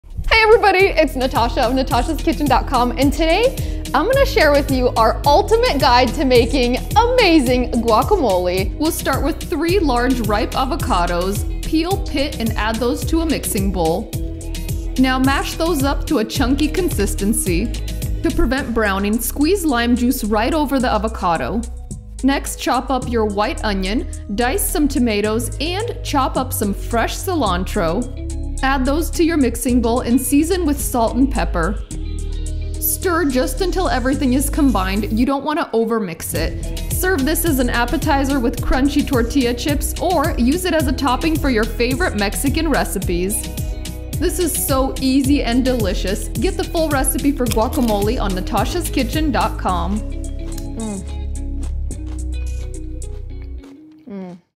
Best Guacamole Recipe
This authentic guac recipe is loaded with avocados, tomato, onion, cilantro, plenty of lime juice, salt, and pepper.